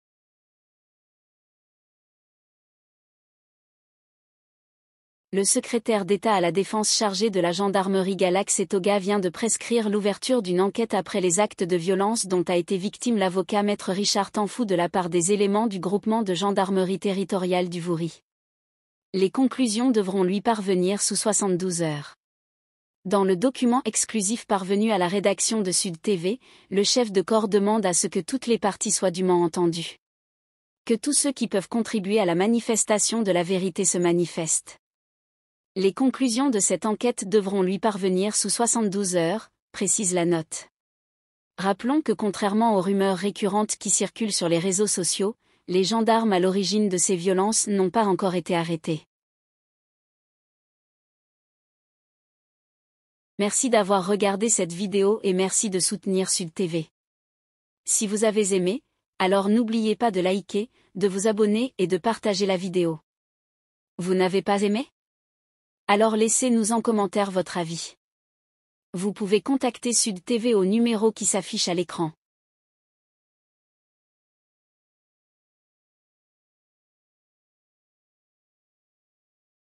AGRESSION DE MAITRE RICHARD TAMFU : LE SED DILIGENTE UNE ENQUÊTE
Le secrétaire d'État chargé de la gendarmerie a prescrit une enquête après les graves violences subies par Me Richard Tamfu

Pour vos couvertures médiatiques, annonce et autres communiqués d’intérêt général contactez Sud Tv  au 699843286.

La télé qui connecte les sudistes
 e télé wongan